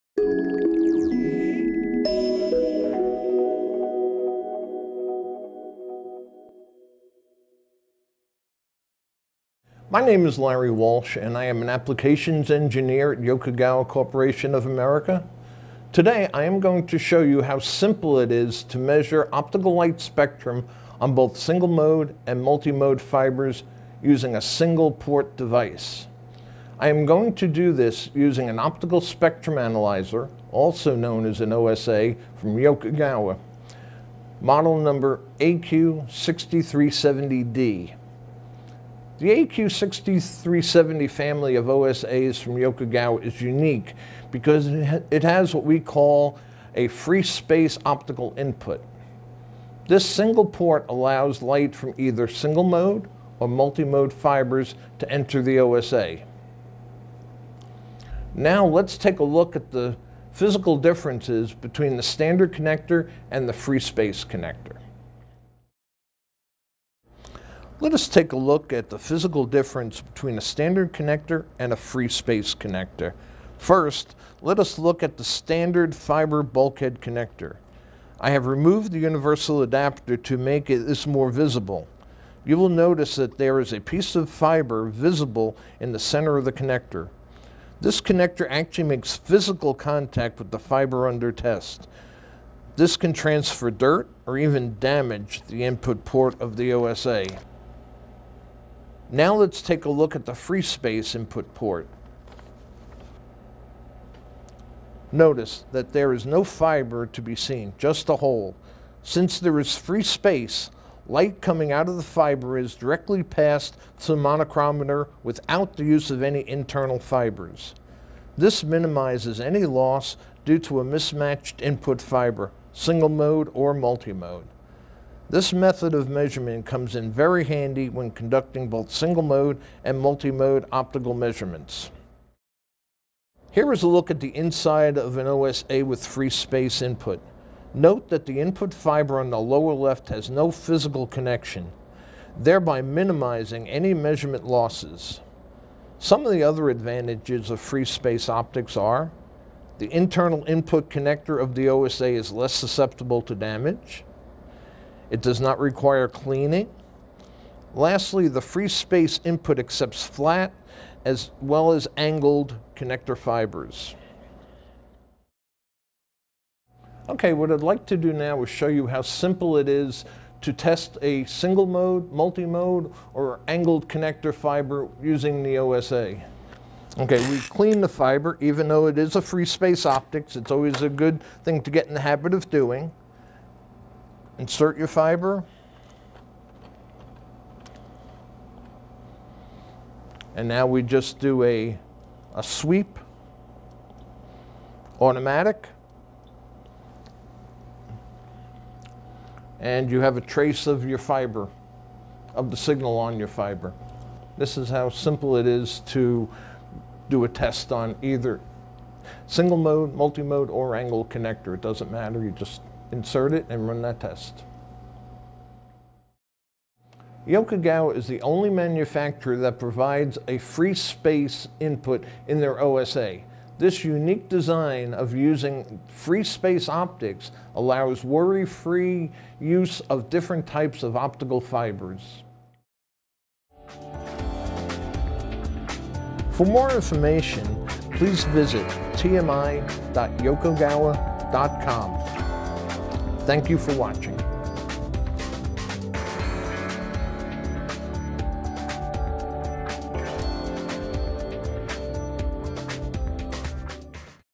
Yokogawa AQ6370D Optical Spectrum Analyzer (OSA)
Deze OSA is geschikt voor het meten in zowel singlemode als multimode toepassingen.